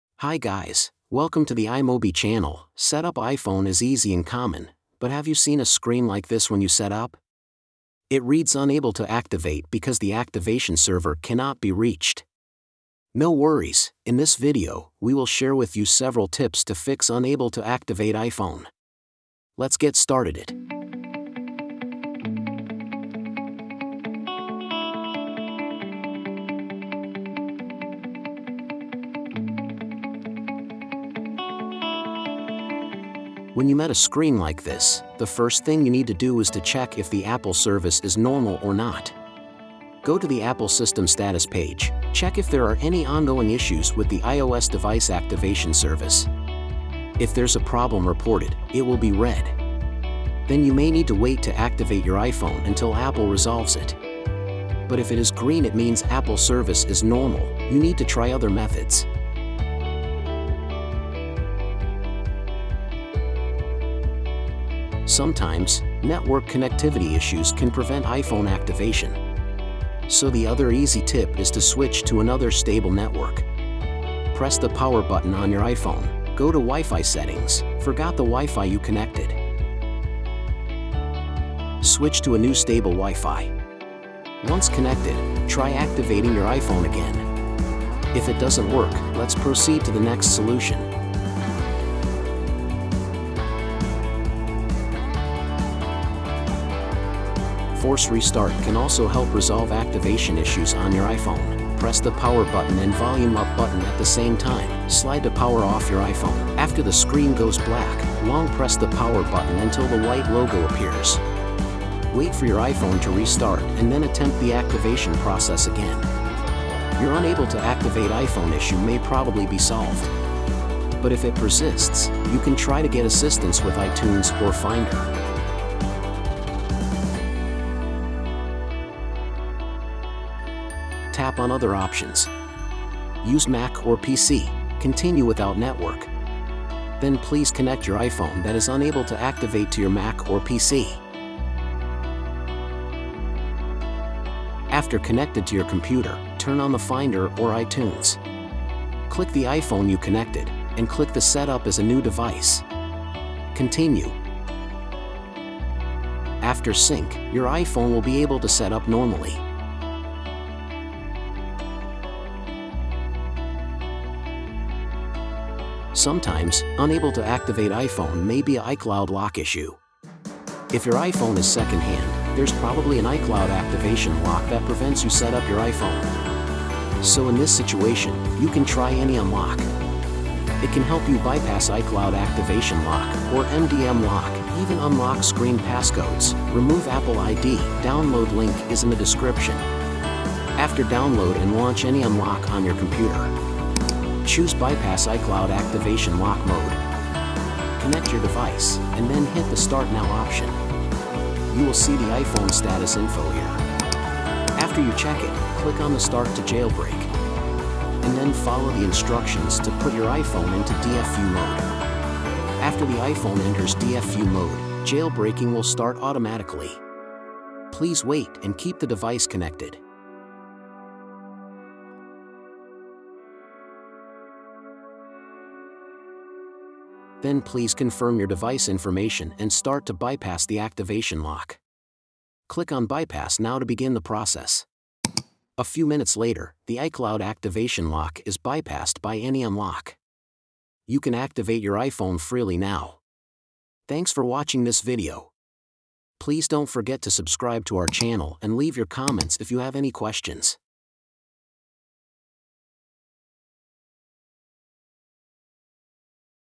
Unable to Activate iPhone? 5 Tips to Fix it Instantly!
Have you met an unable to activate iPhone screen when you set up your iPhone as a new device? Most of the situation is because the activation server cannot be reached.  But No worries! In this video, we will show you 5 tips to solve it.

If you tried to fix unable to activate iPhone with AnyUnlock, then please note that the iCloud Activation Lock Bypass feature only supports iOS 12 to iOS 16.5 on Mac, and supports iOS 12 to iOS 16.4 (iOS 12.4.5 & 12.4.6 excluded) on Windows. And supports the iPhone 5s to iPhone X, iPad 4 to iPad 7, iPod touch 6 to iPod touch 7.
00:00 Intro
00:30 Tip 1. Check System Status
01:02 Tip 2. Change a Network
01:34 Tip 3. Force Restart iPhone
02:08 Tip 4. Activate iPhone with Finder/iTunes
02:42 Tip 5. Fix Unable to Activate iPhone with AnyUnlock